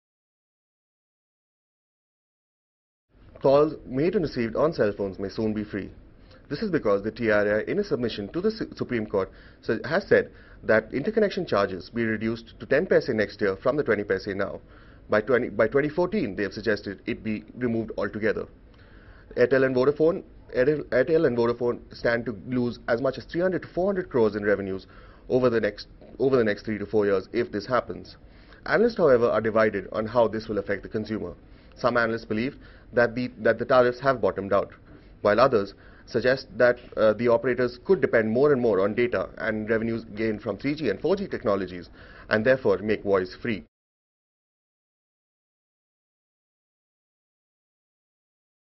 Free calls?
Mint?s Shauvik Ghosh says that while voice calls could soon be free if a Trai recommendation comes into effect, the move won?t be welcomed by many operators.